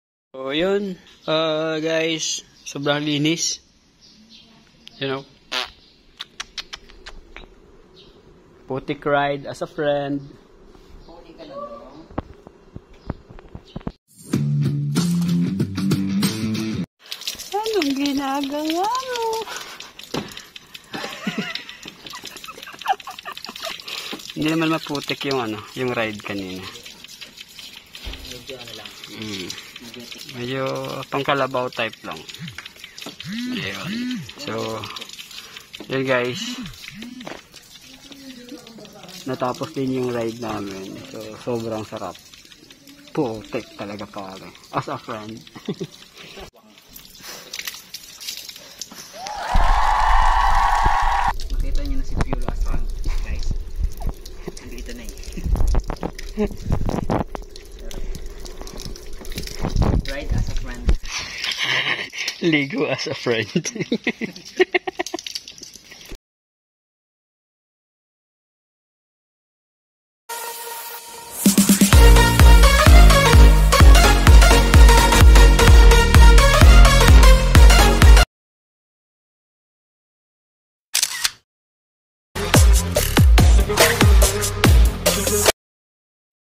bike as a friend last part
tamang ride lang under the rain and a a muddy trail for a friend!!!
got rim cut, snake bite flat in the first round karma nga talaga!!